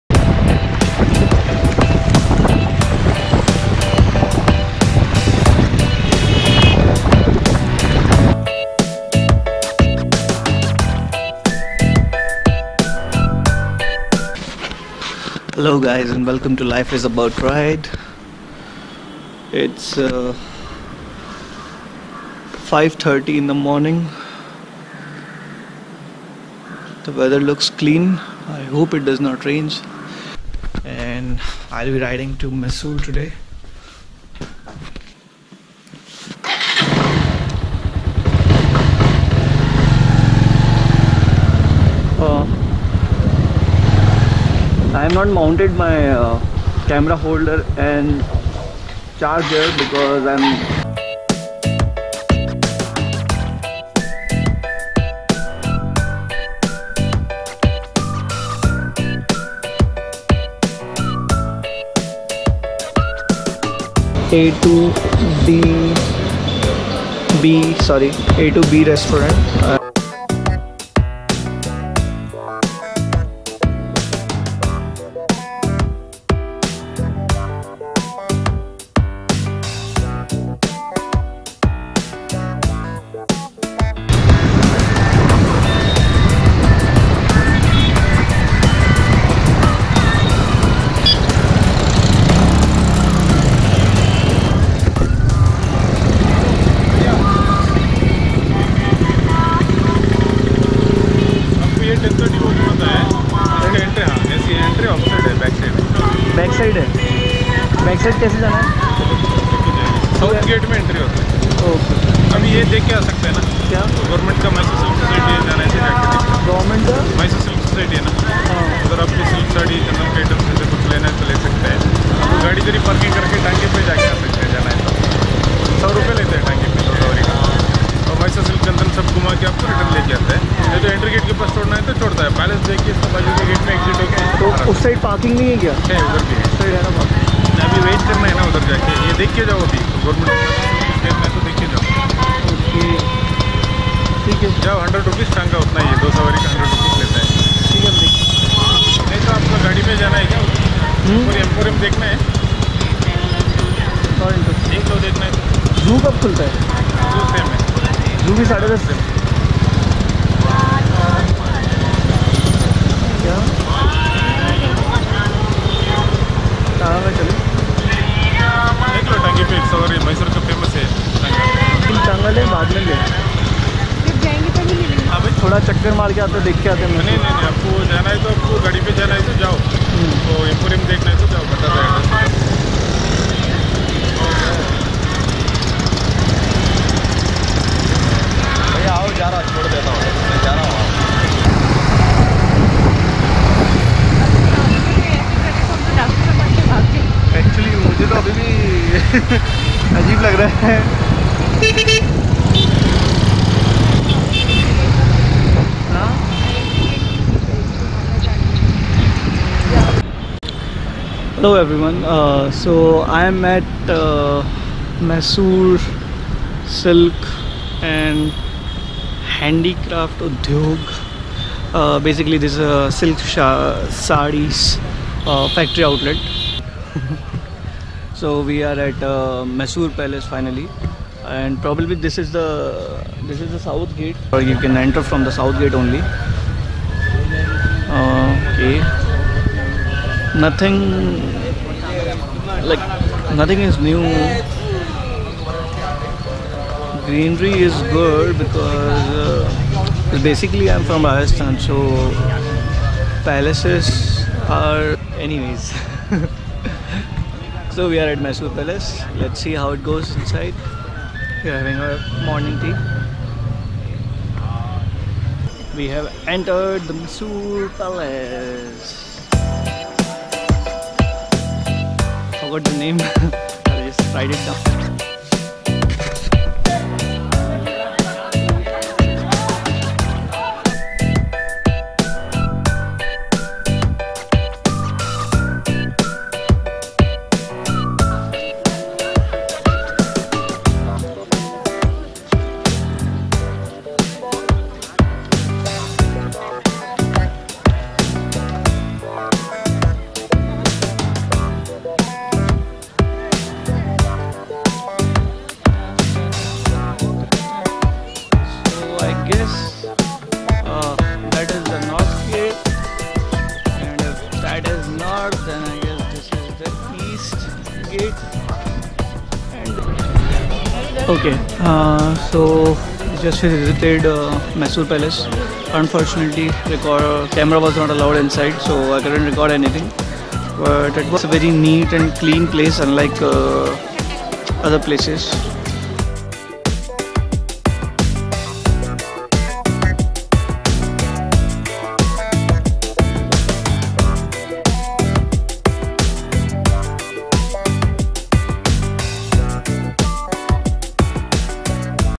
Mysore Palace | Short Ride | LIA Ride
One day trip to Mysore, visited the famous mysore palace and the zoo however this video is only about Mysore palace.

Gears I use:
Cam: GoPro Hero 5
Gloves: Vega
Helmet: Studdds